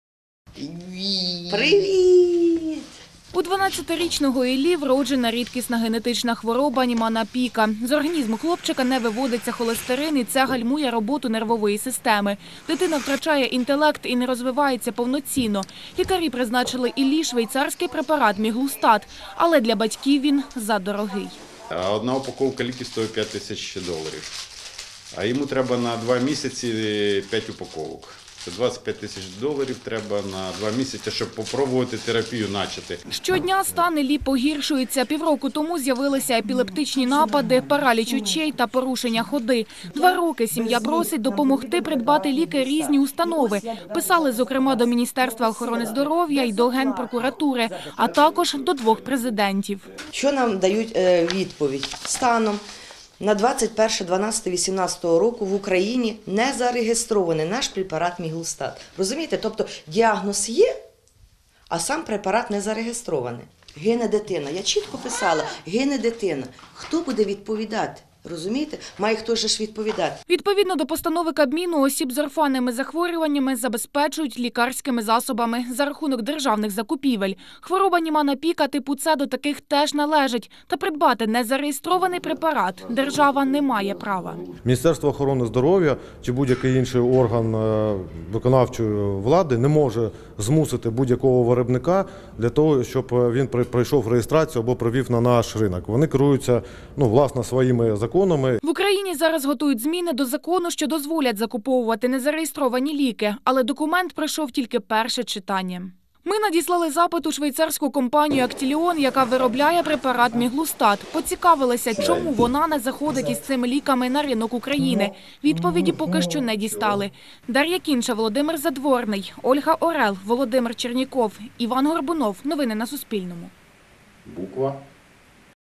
29.02.20. 12-річний вінничанин Ілля хворіє на рідкісну генетичну недугу
У дванадцятирічного Іллі  з Вінниці  рідкісне генетичне захворювання Німана-Піка (С). З організму не виводиться холестерин, і це гальмує роботу нервової системи. За 2 місяці на лікування родина витрачає 25 тисяч доларів, каже батько хлопчика Віталій Верховний. Ліки  пацієнти з рідкісними недугами,  за законом мають отримувати безкоштовно через державні закупівлі.  Сьогодні, у  міжнародний день рідкісних захворювань, Дар'я Кінша розкаже чи  діє закон насправді.